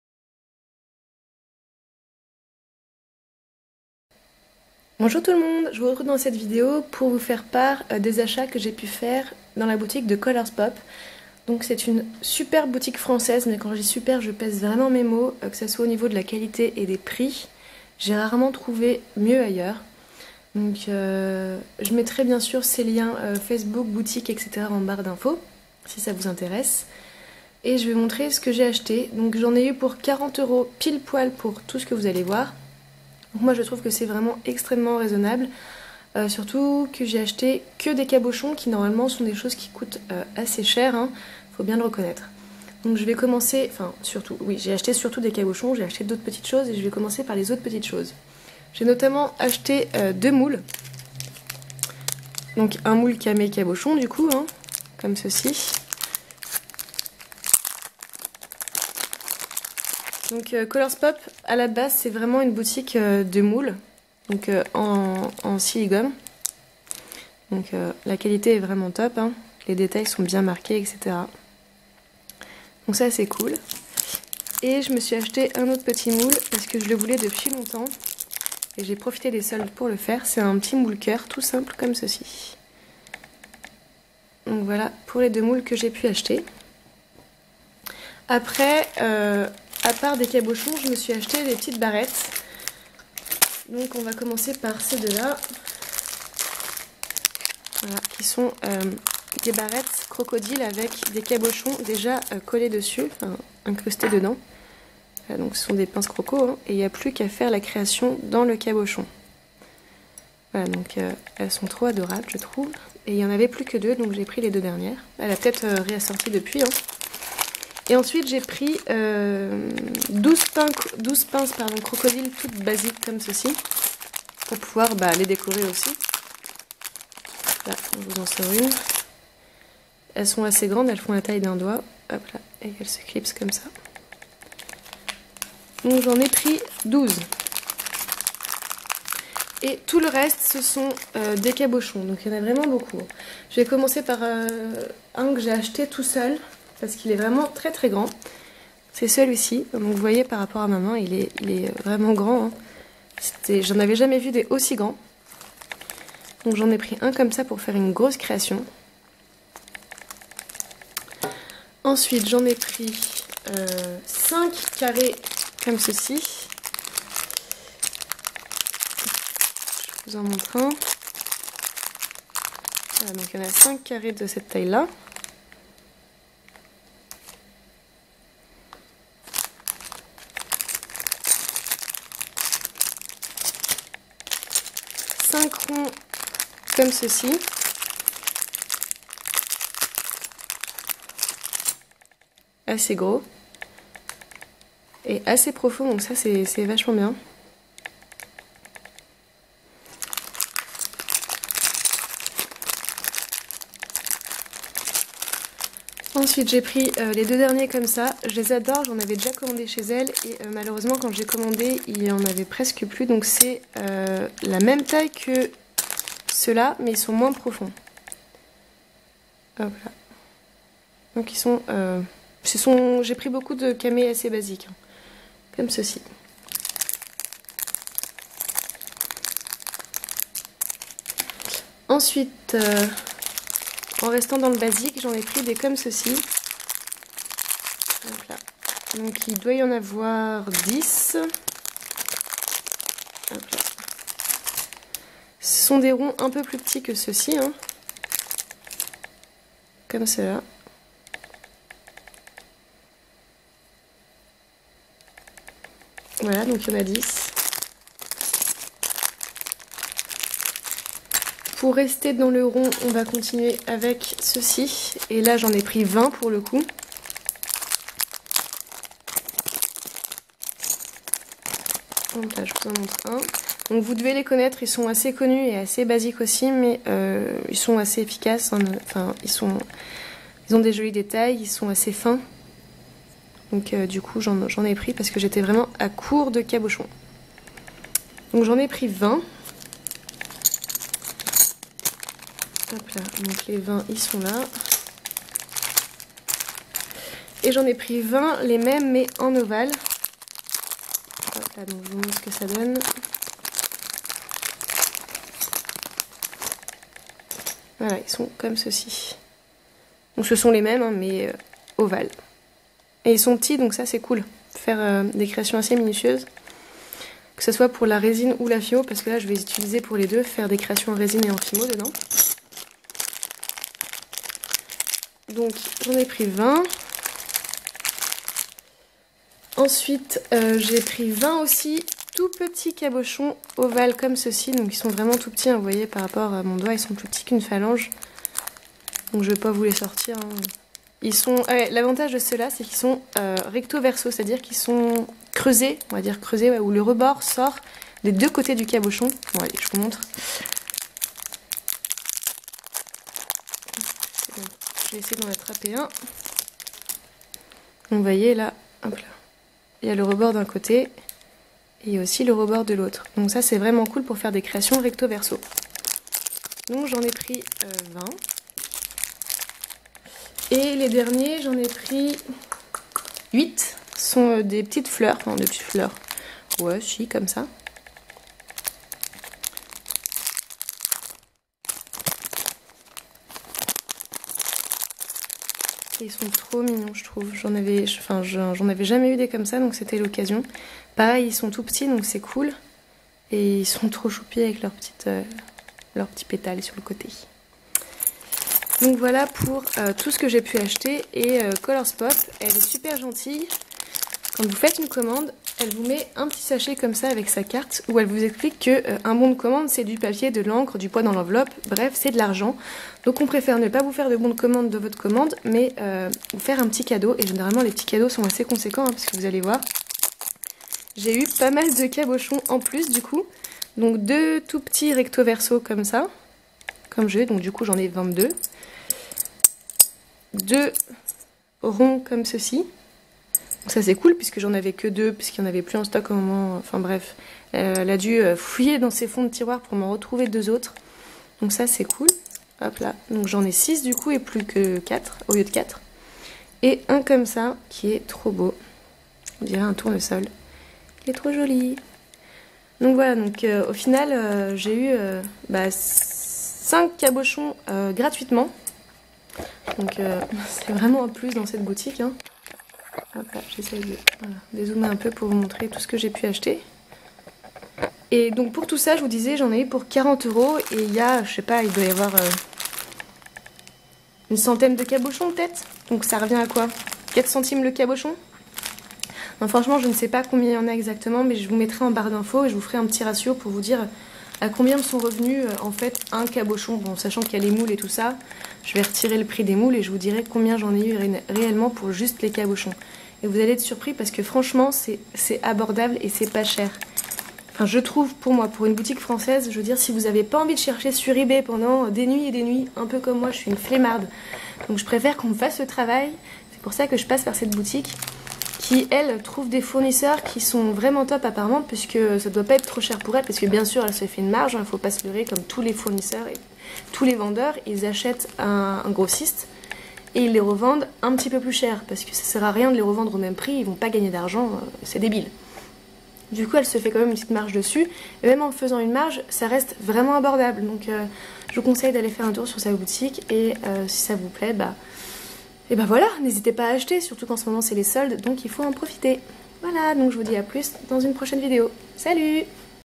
[ACHATS] ColorsPop
Voici les achats que j'ai effectué sur la boutique ColorsPop!